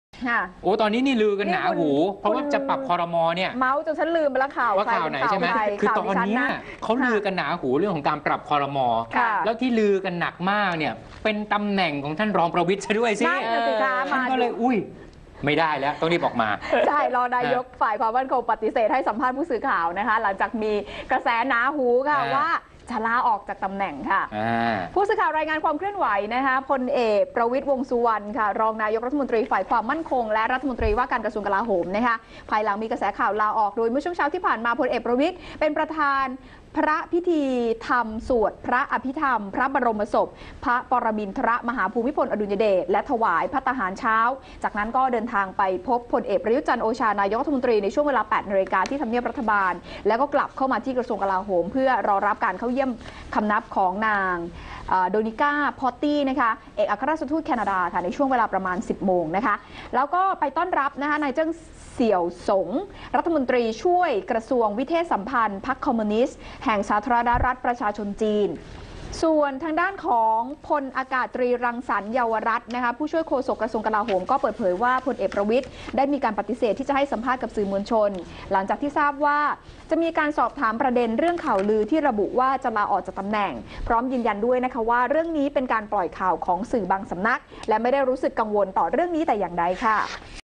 "ประวิตร" ไม่พูดเรื่องข่าวลาออก
16/12/2016 "ประวิตร" ไม่พูดเรื่องข่าวลาออก